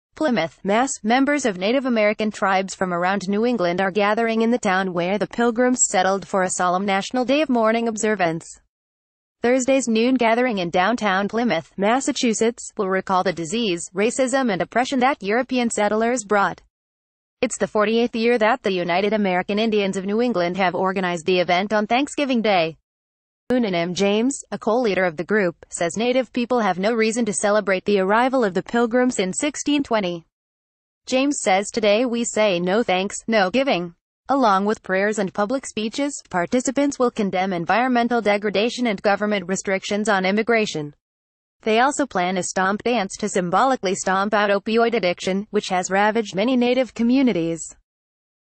The Fox News - Native Americans marked Thanksgiving Day of mourning
The Fox News - The Fox News - Native Americans marked Thanksgiving Day of mourning

PLYMOUTH, Mass. –  Members of Native American tribes from around New England are gathering in the town where the Pilgrims settled for a solemn National Day of Mourning observance. Thursdays noon gathering in downtown Plymouth, Massachusetts, will recall the disease, racism and oppression that European settlers brought. Its the 48th year that the United American Indians of New England have organized the event on Thanksgiving Day. Moonanum James, a coleader of the group, says native people have no...